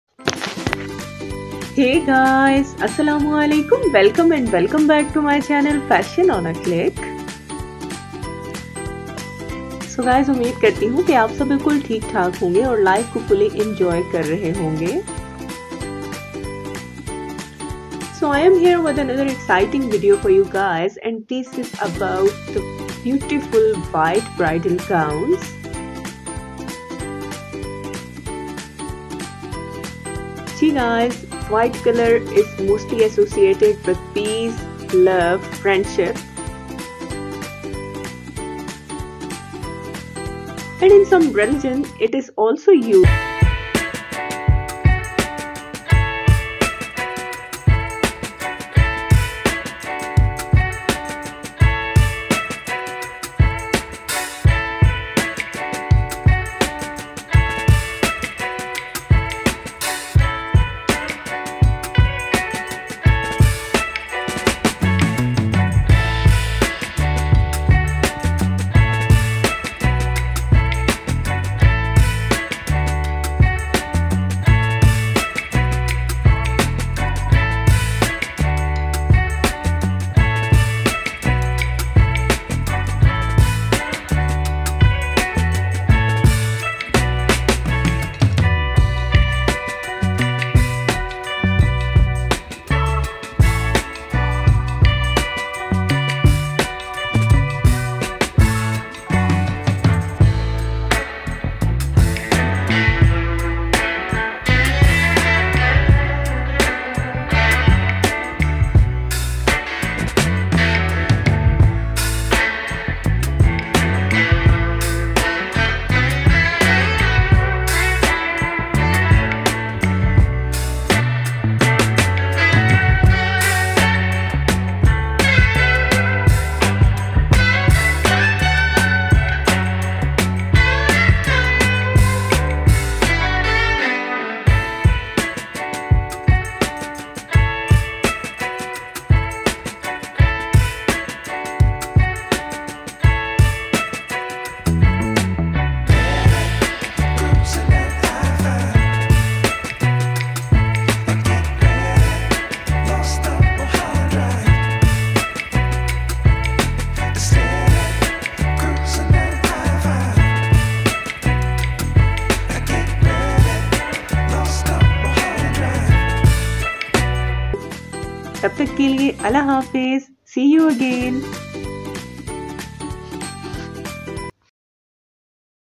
white bridal gowns latest#2022#beautiful
This video is about white bridal gowns latest#2022#beautiful.
The video shows highly rich ideas of beautiful white wedding gown designs and unique patterns.
These white bridal gowns are best for girls searching for a white heavy maxi or gown dress.
These white bridal gowns are also best for brides' sisters, cousins, and friends for wearing at reception parties.
School and college-going girls would also love to wear these white gowns at their parties and gatherings.
White color is known for beauty, friendship, and peace. These white bridal gowns will give you a stunning look .choose a design of your choice show it to your designer and get it In the form of a dress.
White Is a soft and soothing color that spread freshness to our eyes so when we find a bridal dress and party theme of white color it looks beautiful and heart taking.
These beautiful white bridal dress ideas will touch your heart.
hoping for your positive consideration.
white bridal gown.
latest white bridal gowns.
#2022 bridal gowns.
beautiful bridal gowns.